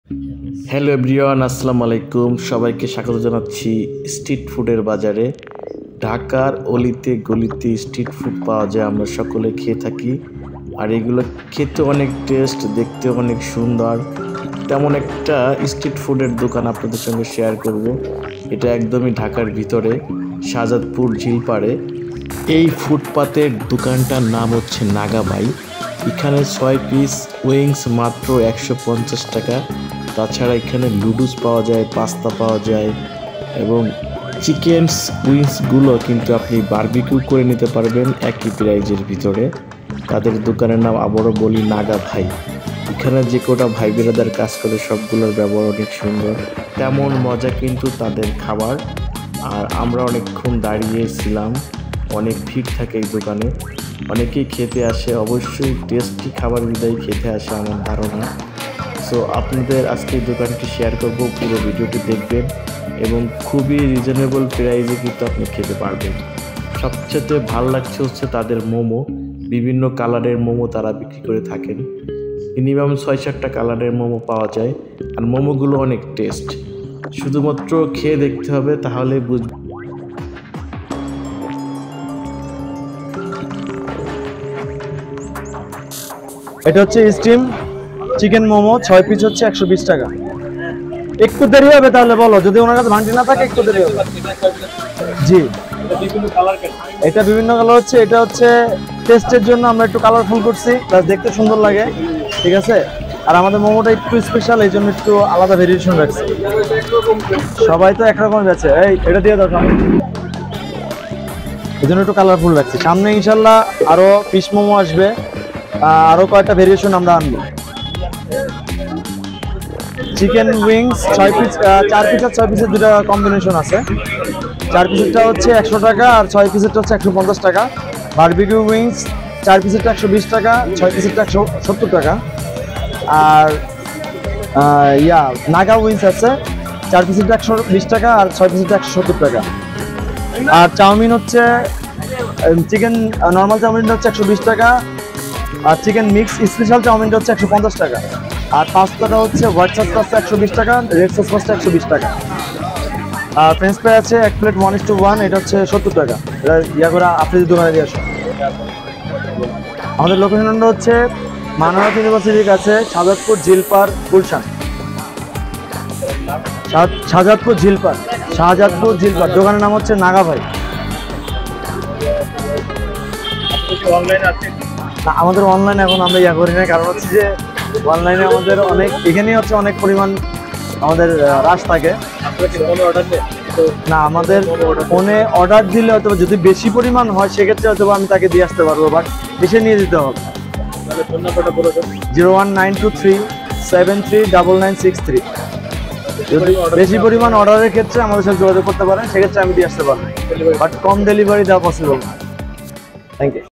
bangladeshi street food||Naga Bhai।। ছয় পিচ চিকেন মাত্র ১৫০ টাকা
শাহজাদপুর চিল পারে রোডের উপরে এই দোকান নাগা ভাই
তারা খুবই অল্প দামে চিকেন ফ্রাই,পাস্তা, লুডুস, মম, সাব স্যান্ডউইচ, বার্গার,ইত্যাদি বিভিন্ন প্রকার খাবার বিক্রি করে থাকেন
খুবই সুস্বাদু এই খাবার